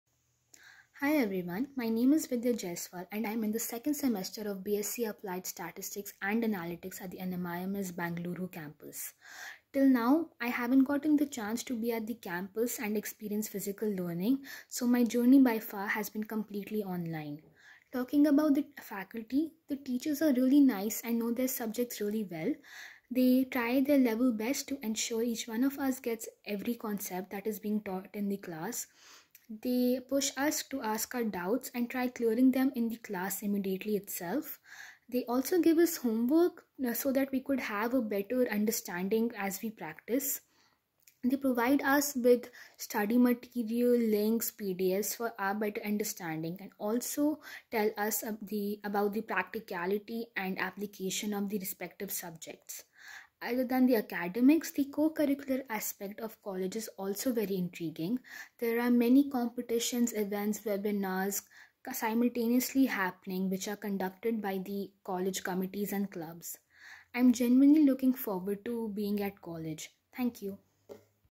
NMIMS Vidya Jaiswal
What is it like being a part of NMIMS during the pandemic? Listen to our 1st year B.Sc. (Applied Statistics and Analytics) student, Ms. Vidya Jaiswal, share how she hasn't been missing out with the NMIMS School of Science, Bengaluru, taking its classes and campus life online.